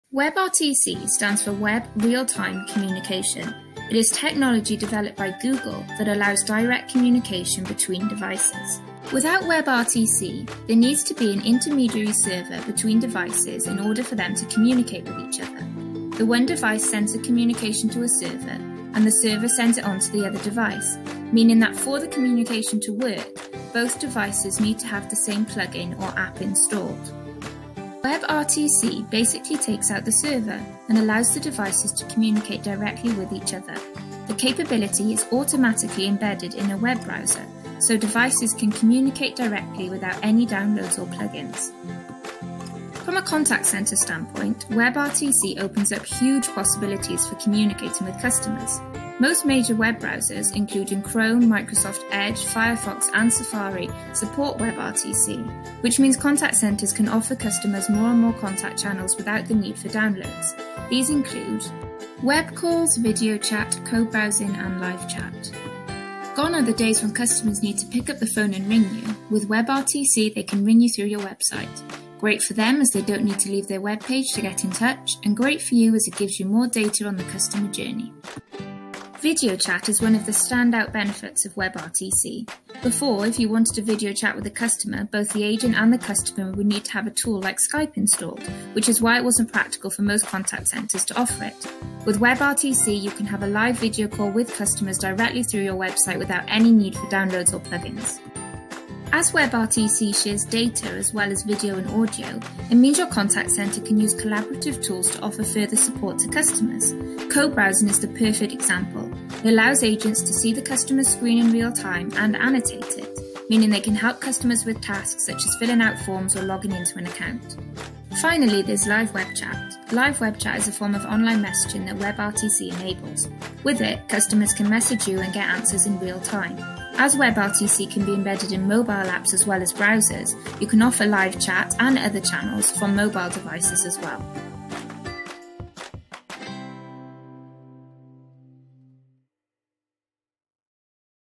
What is WebRTC and how is it useful in online customer service?
WebRTC has changed the face of online customer service. It's the technology that allows businesses to offer web calling, live chat, cobrowsing and video chat all through their website.

Understand how WebRTC works and how it helps deliver superior online customer support to your customers!